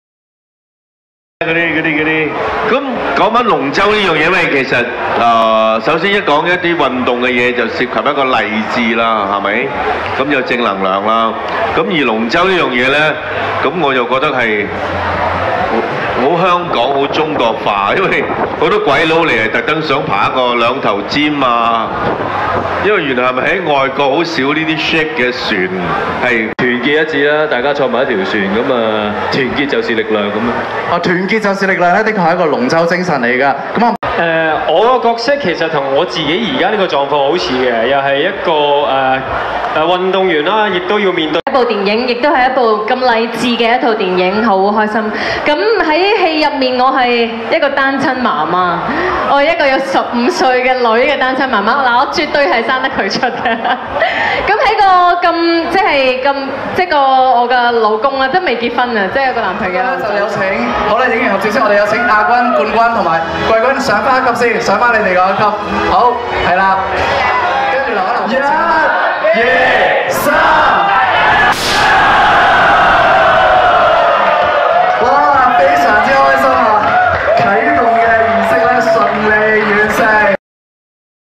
陳詠燊、吳鎮宇、胡定欣、黃德斌、胡子彤《逆流大叔》宣傳啟動禮
今天太陽高掛，一眾嘉賓：陳詠燊、吳鎮宇、胡定欣、黃德斌、胡子彤齊集於中環海濱活動空間主舞台參加《逆流大叔》宣傳啟動禮